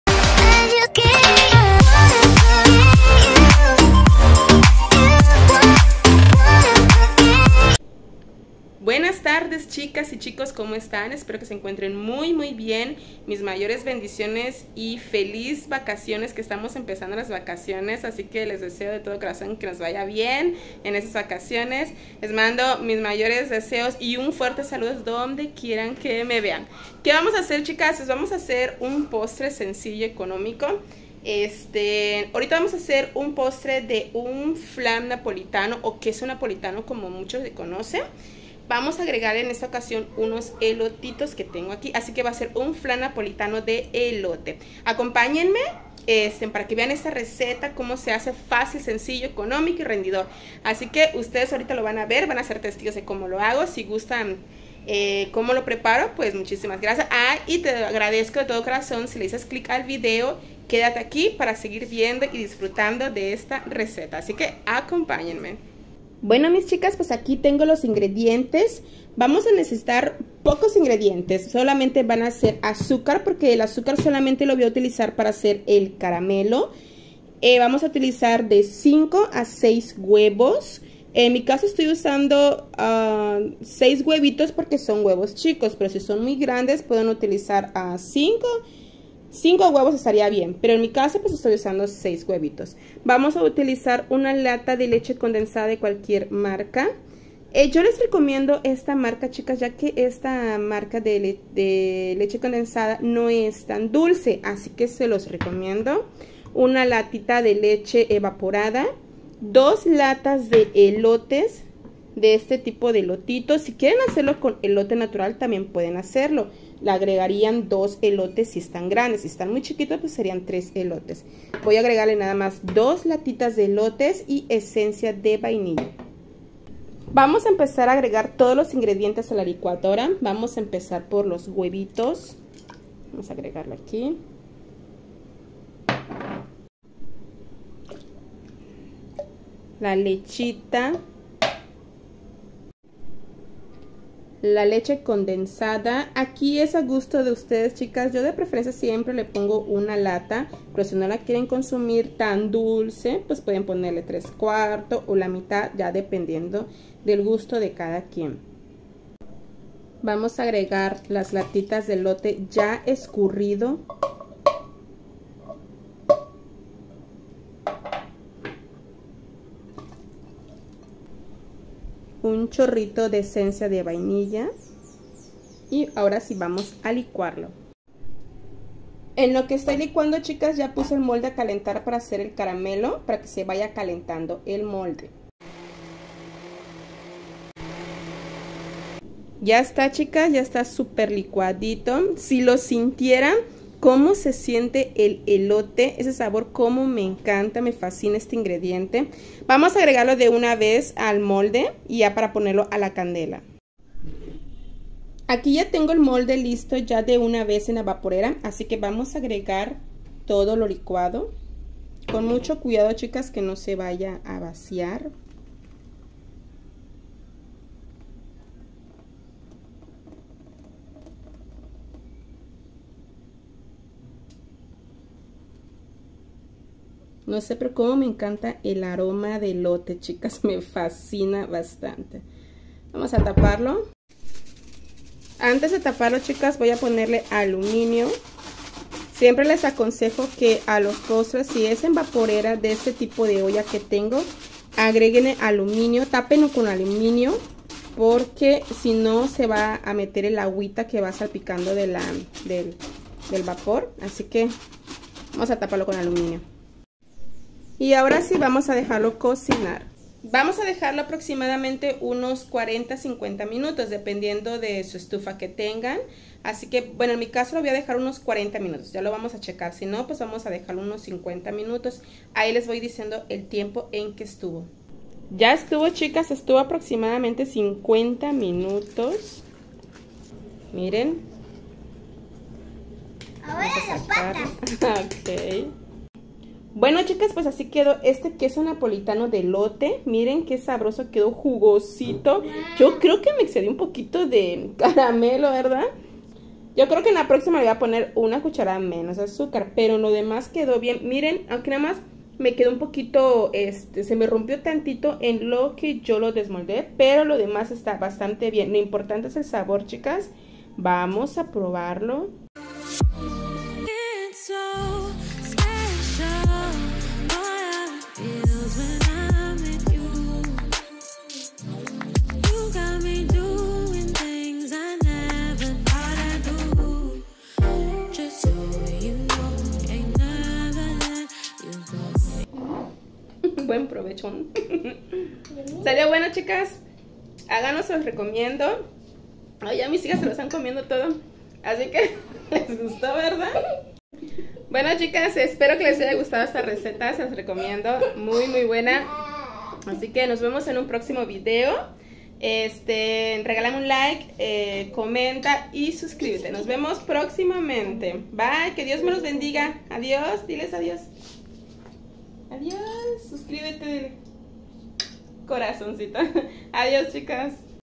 flan napolitano con elote 🍮🌽😋
bienvenidos aquí les traigo un postre de flan napolitano con elote esperando que sea de su agrado 🤗